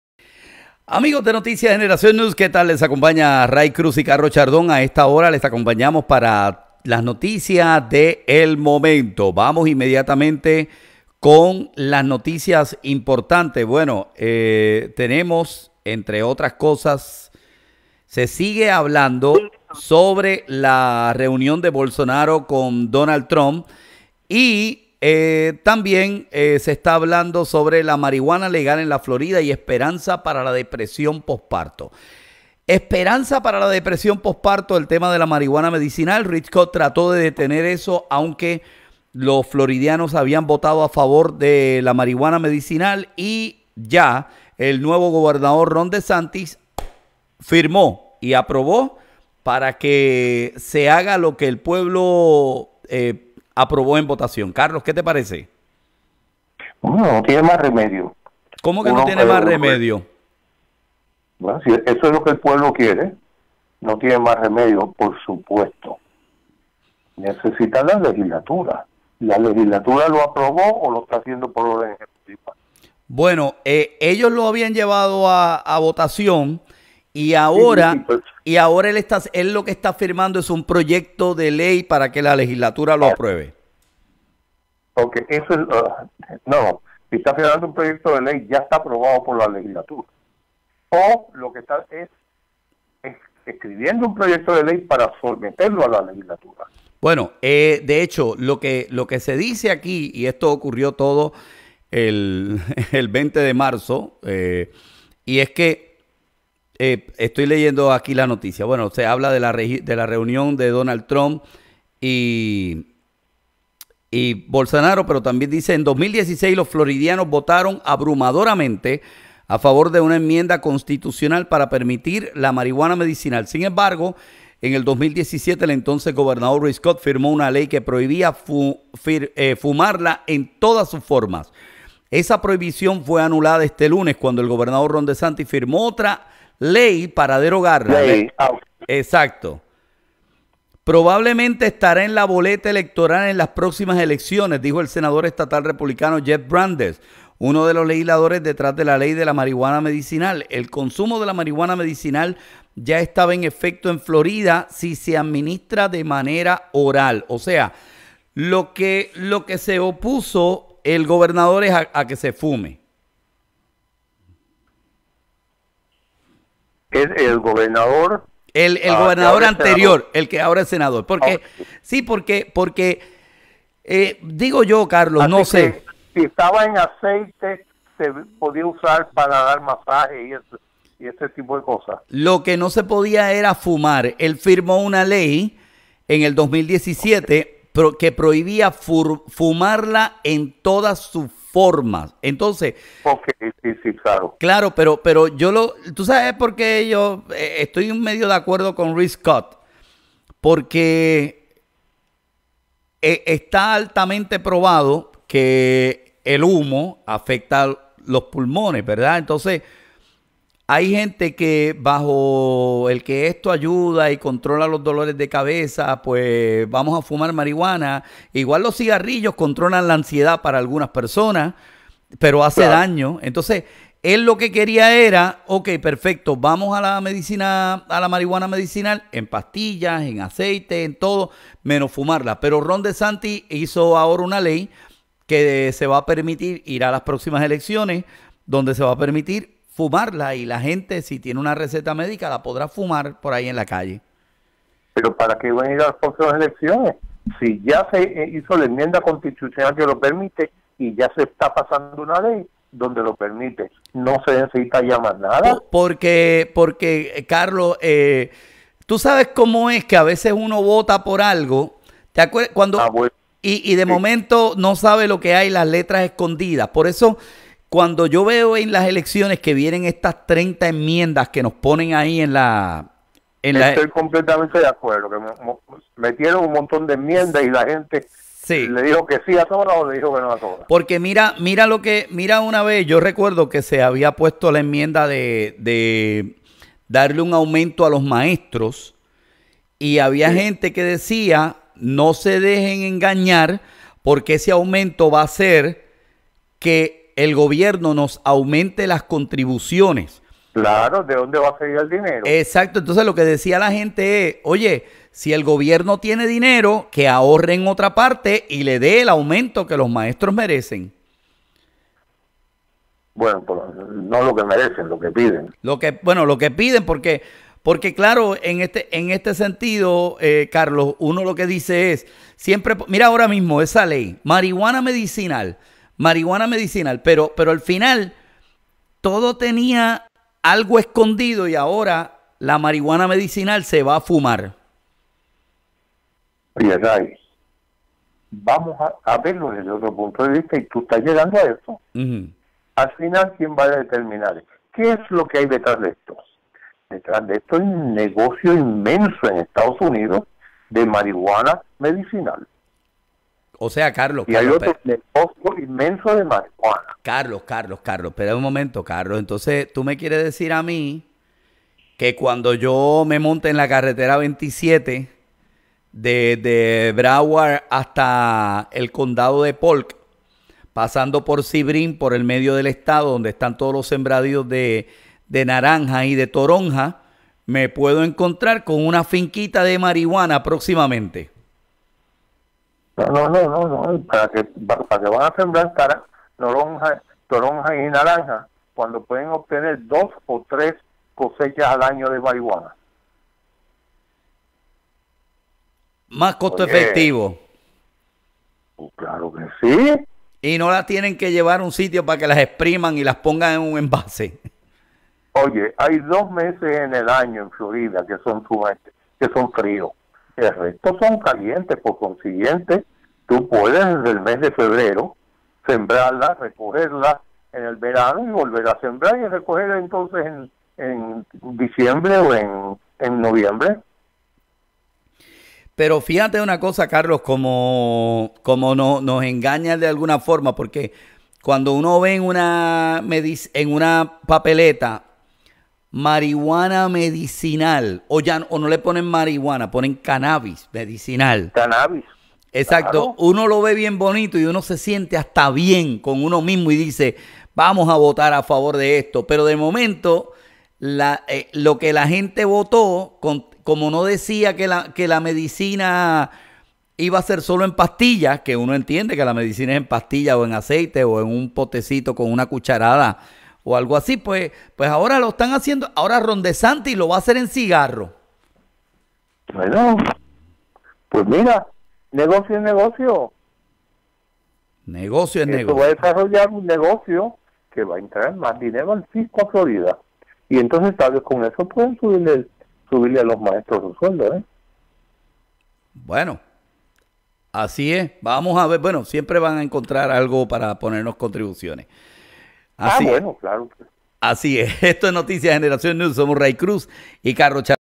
A fumar Marijuana en Florida
El lider conservador Ron DeSantis da un giro a la izquierda al respaldar que los floridianos puedan fumar marijuana libremente